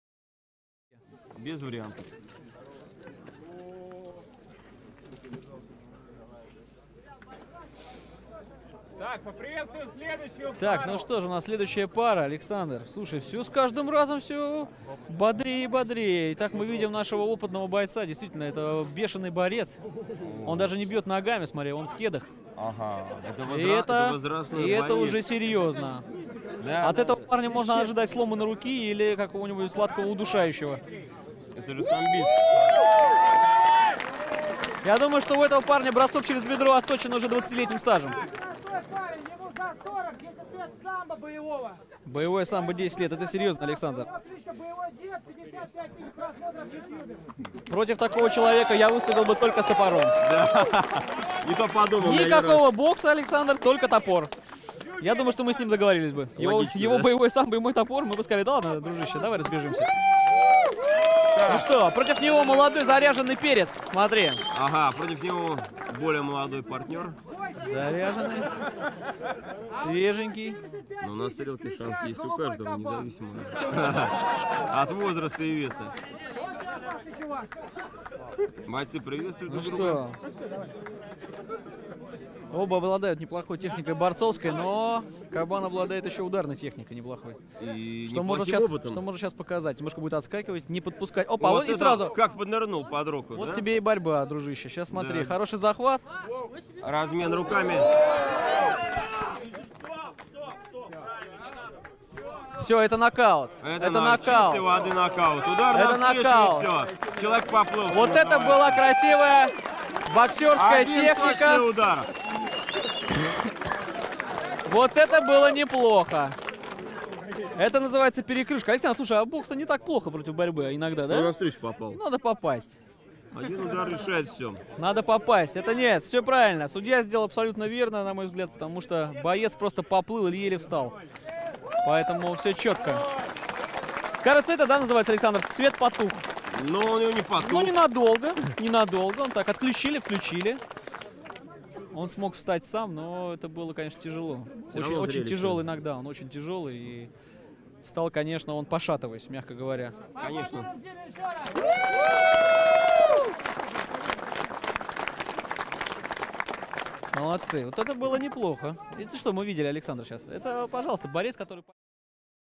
Бой профессионала улицы с опытным борцом
Полученный опыт может пригодится в любых условиях.